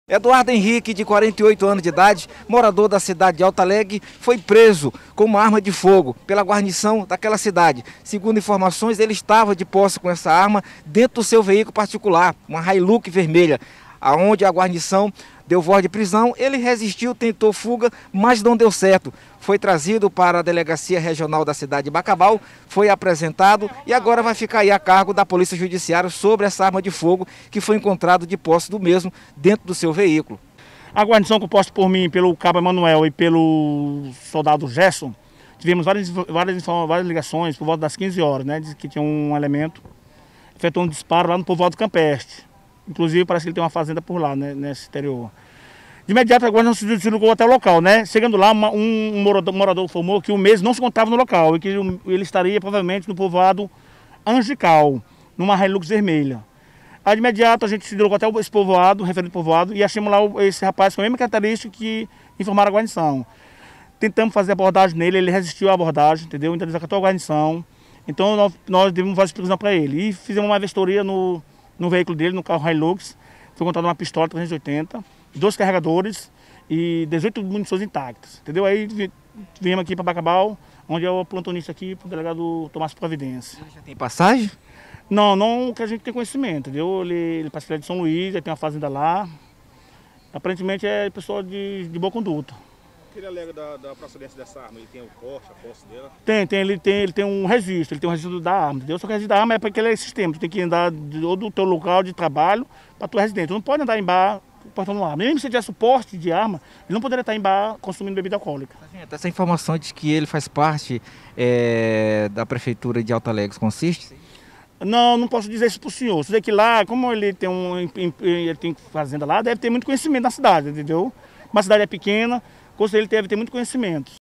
PM TIRA PISTOLA 380 DE CIRCULAÇÃO
FATO ACONTECIDO NA CIDADE DE BACABAL MARANHÃO

Siga a gente no Insta: @bacabalflix